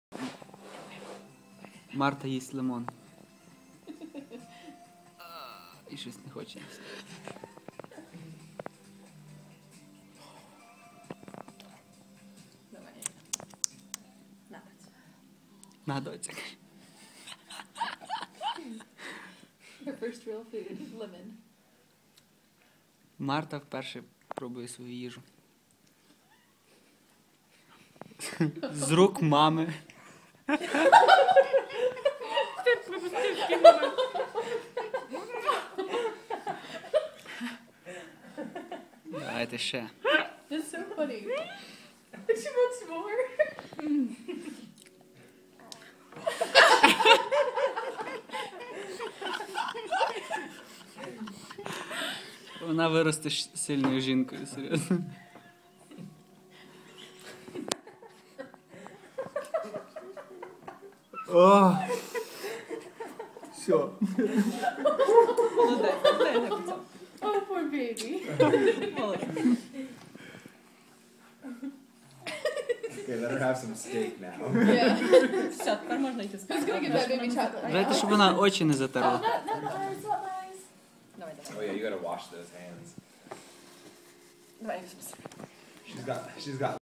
Marta's first solid food - lemon
Because this is Ukraine.